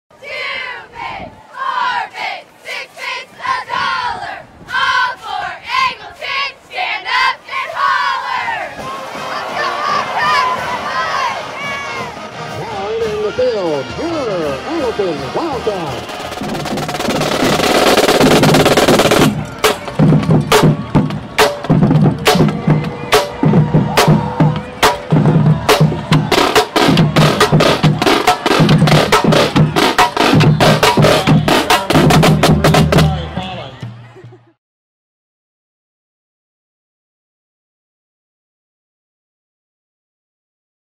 Angleton vs Hightower 2018
The Wildcats notched a win, beating Hightower 14-13 at home!